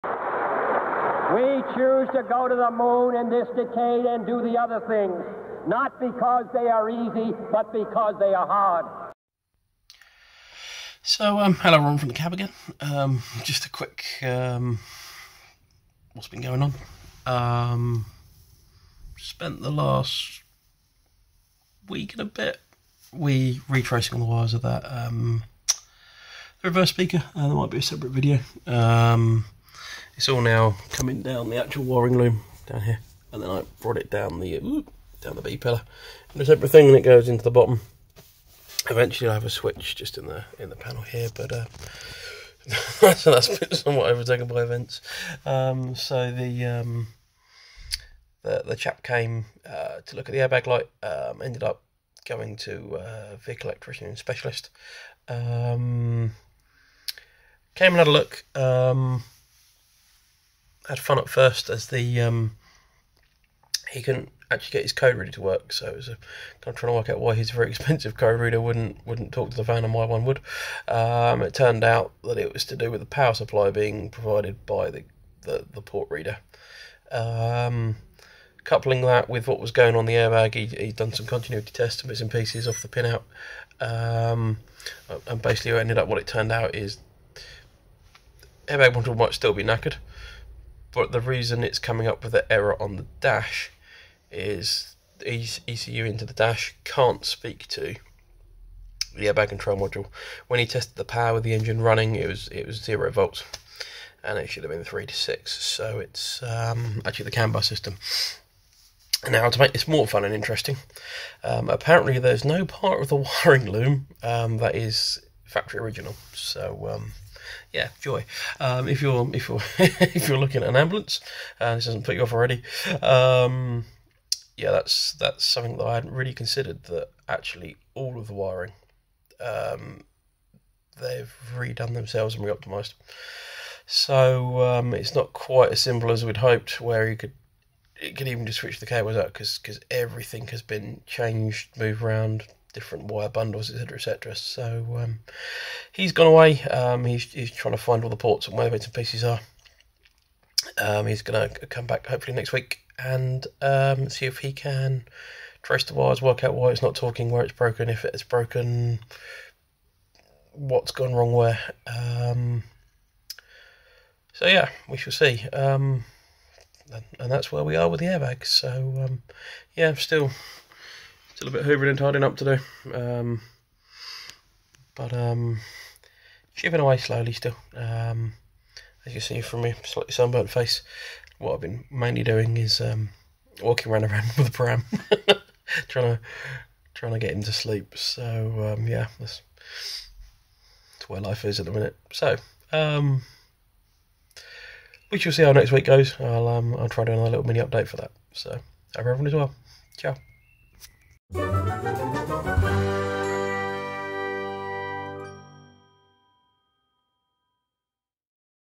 Episode 49 - Airbag Update (Mini Update)
Just a quick update on where we are with the Airbag issue that has been holding the van up for a while after a visit from the Vehicle Electronics Specialist this week.